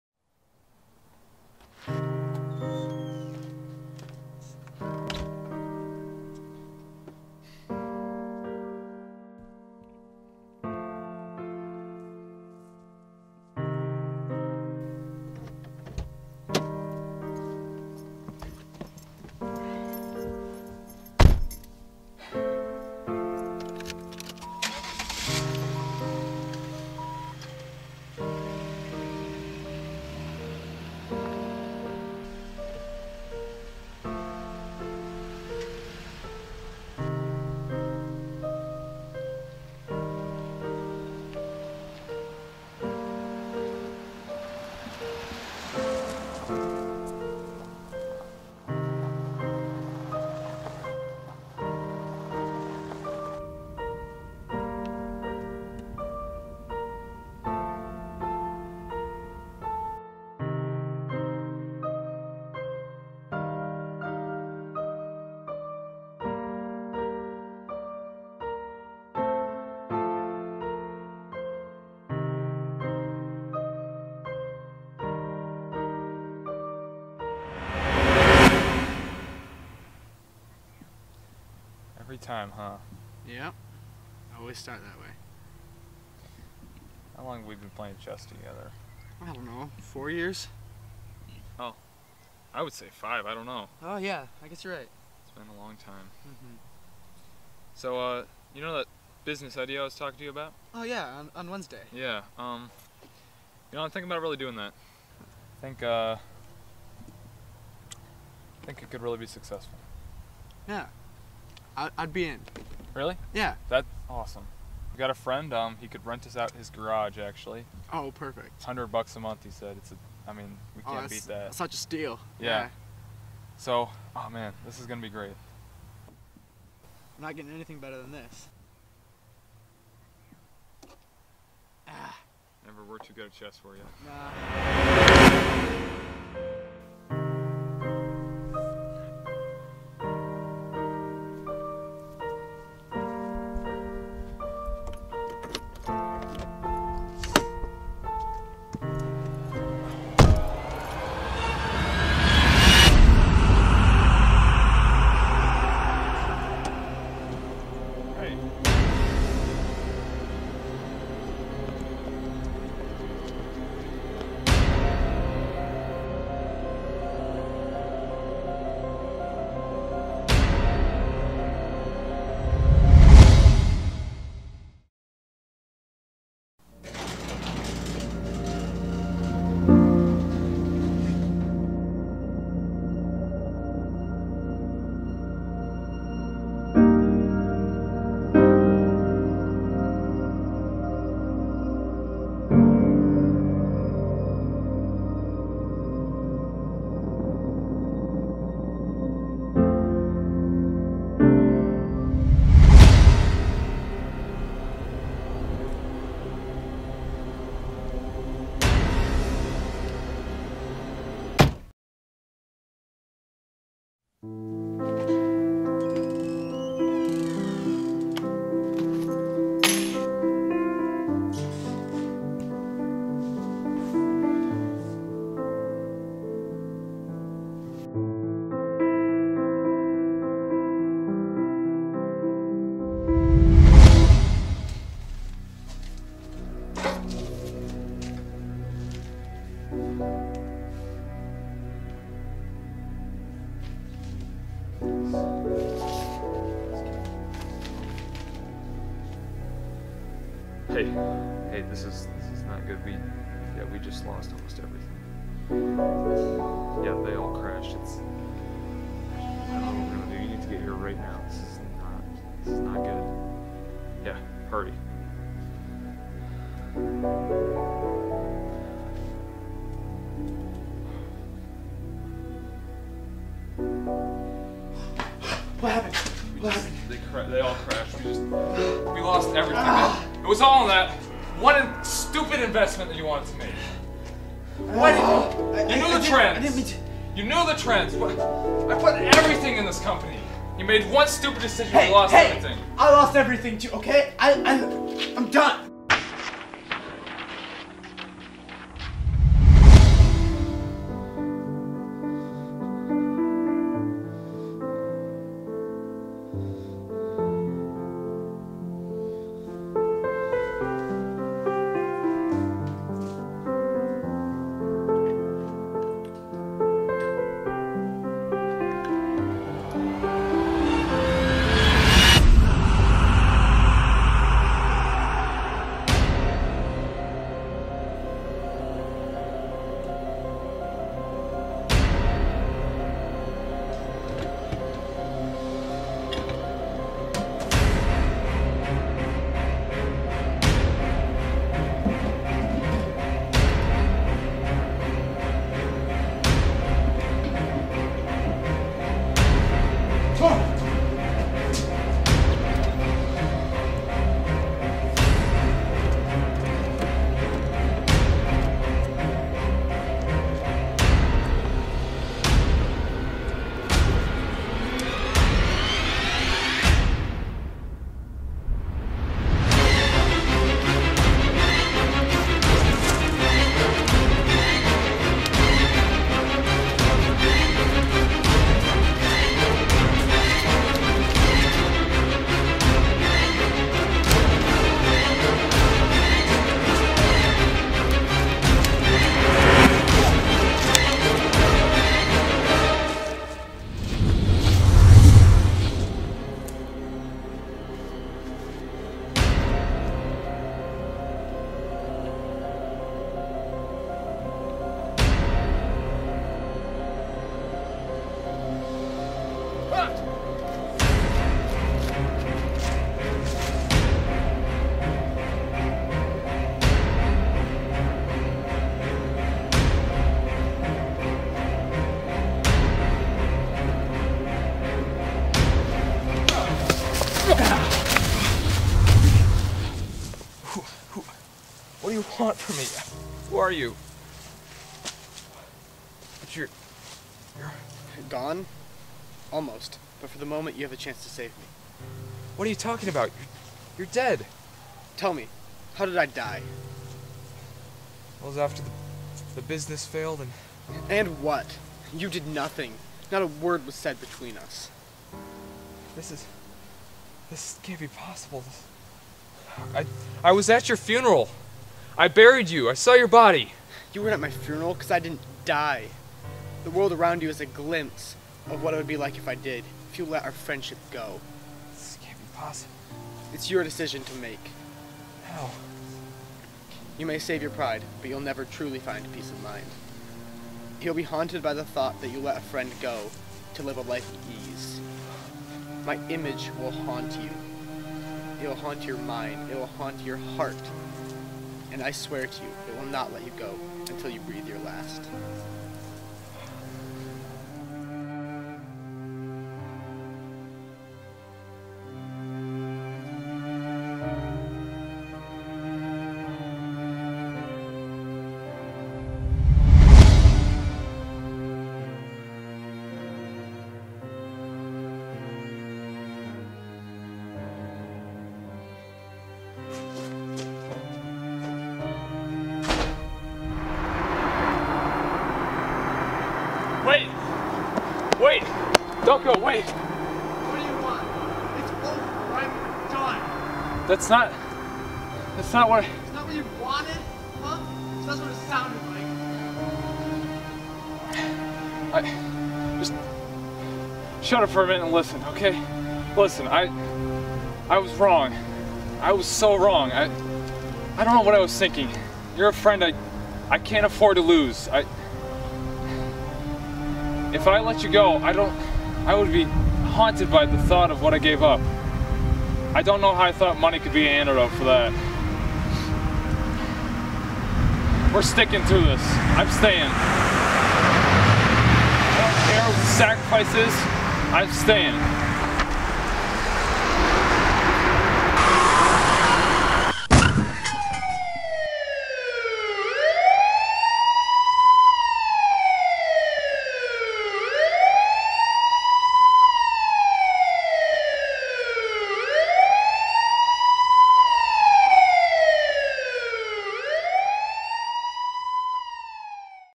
Blink: A Short Film (Adam Young Scores Short Film Contest)
Produced for the Adam Young Scores Contest
Directed and Edited by Brant Meyer
Actors:
Jacob Aupperlee
Jacob Doorn